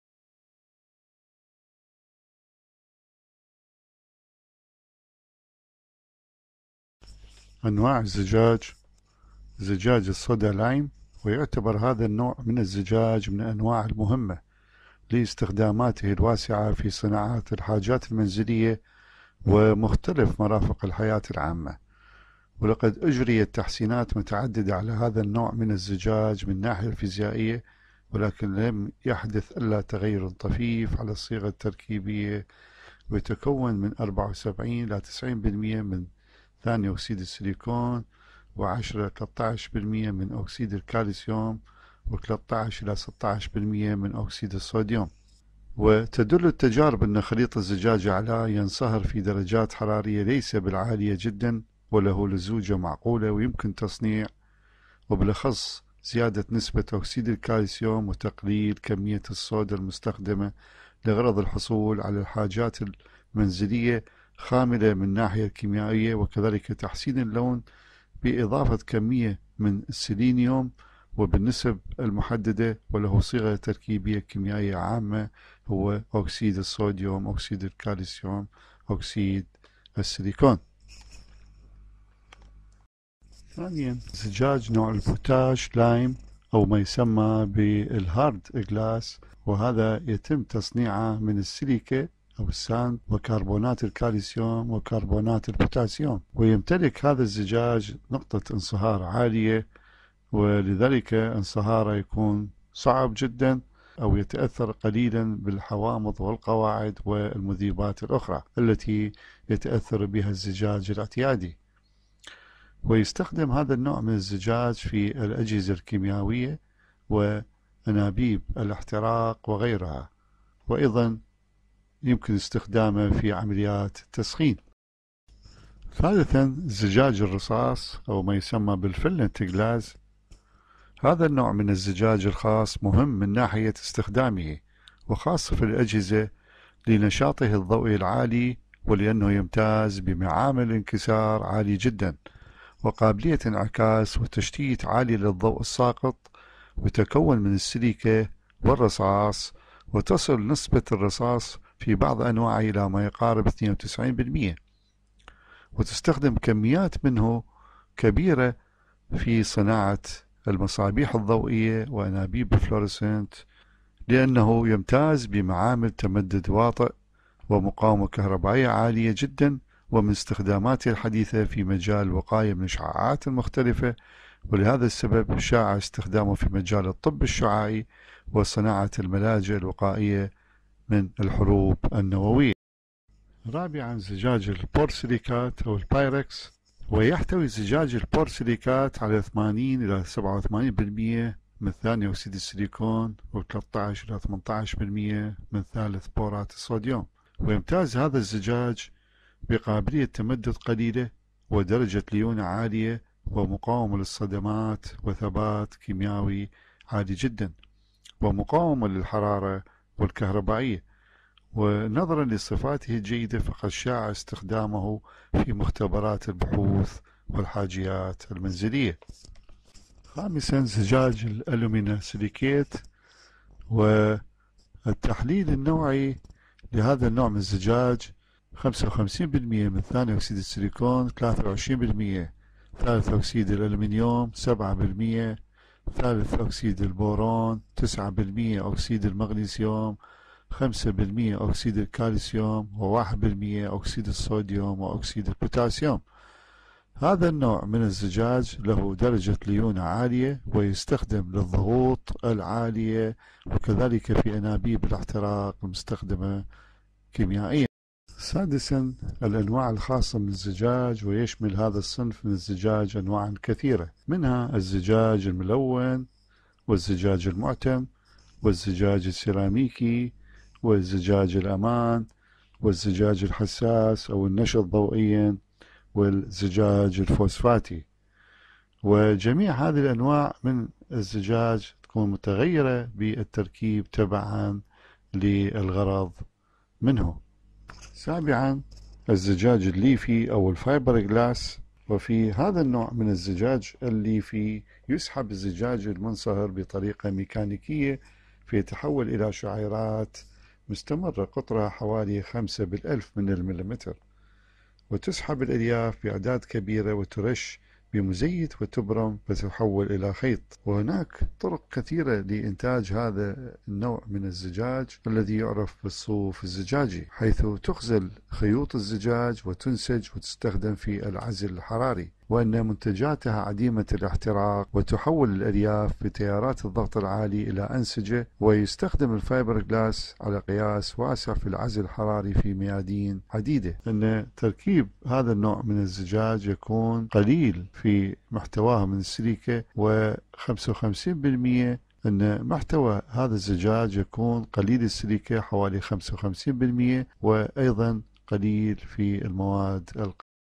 8 Glass industries lec 8 part 2 Adel Sharif
الهندسة الكيمياوية
فرع العمليات الكيمياوية
المرحلة الثالثة
عمليات الصناعات الكيمياوية chemical process industries
استاذ مساعد عادل شريف حمادي